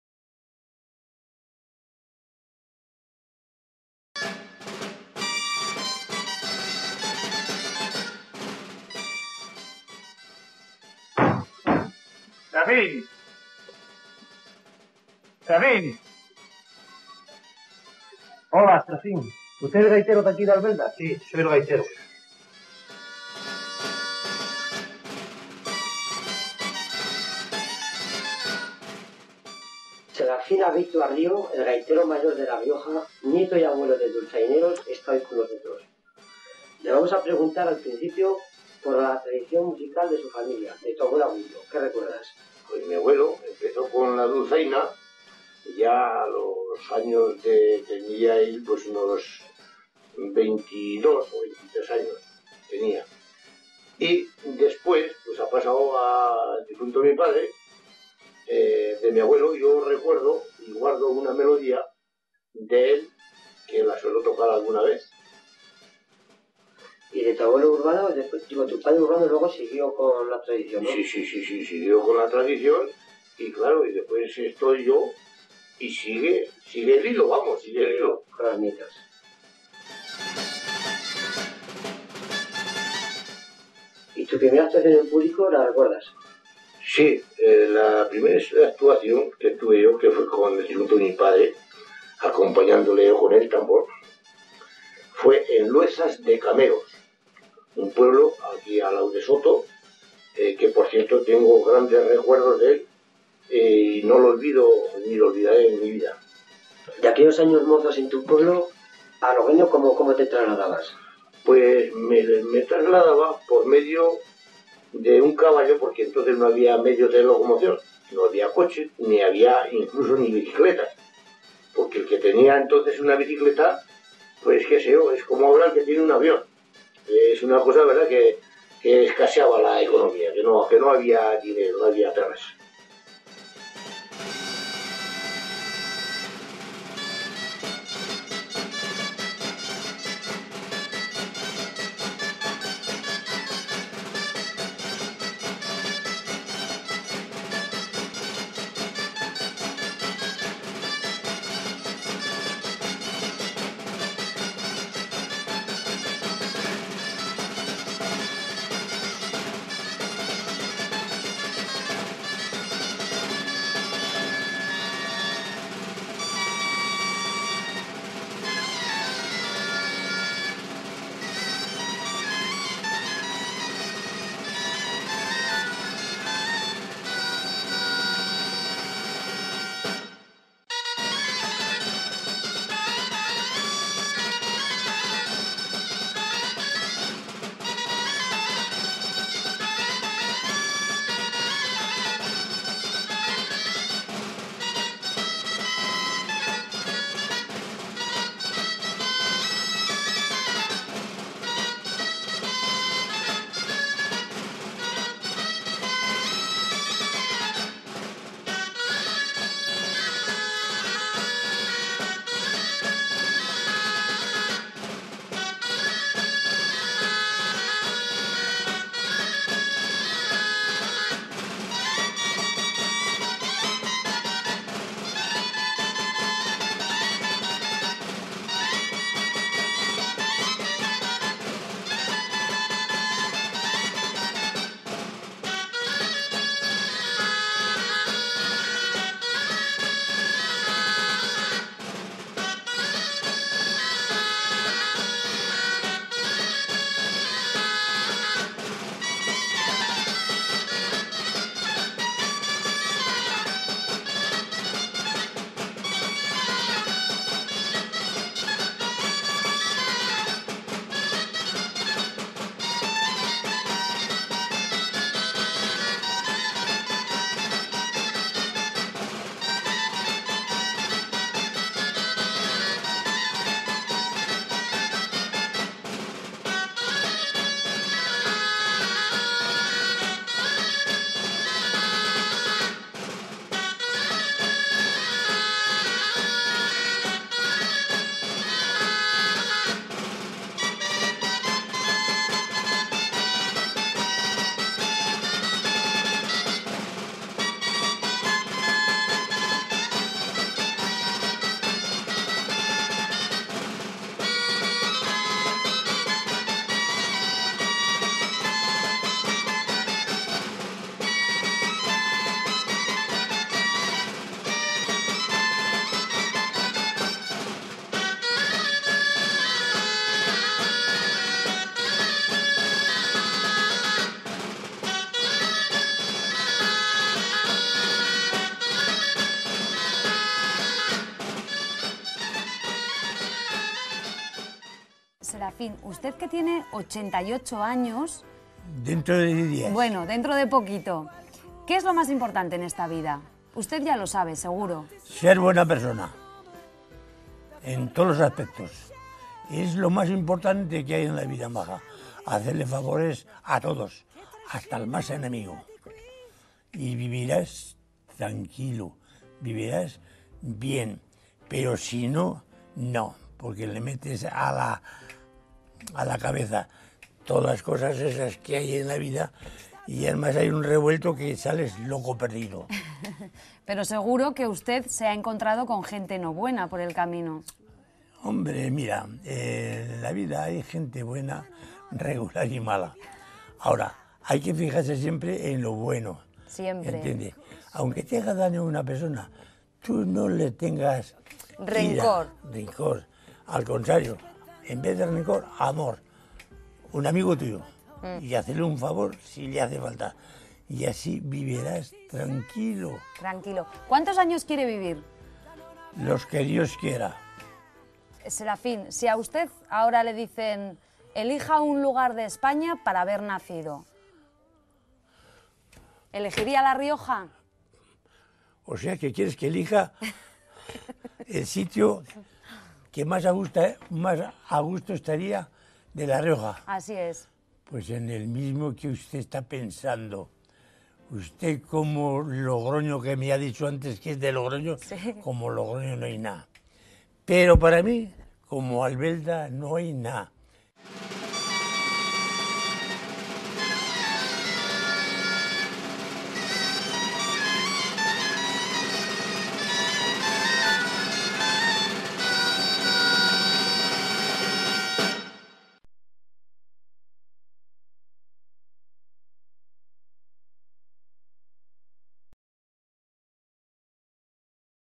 Serafín Abeytua, video homenaje
Video emitido en los homenaje al gaitero de Albelda de Iregua del 18 de julio de 2009 y del 6 de octubre de 2012.
Autor: familia de Serafín Abeytua.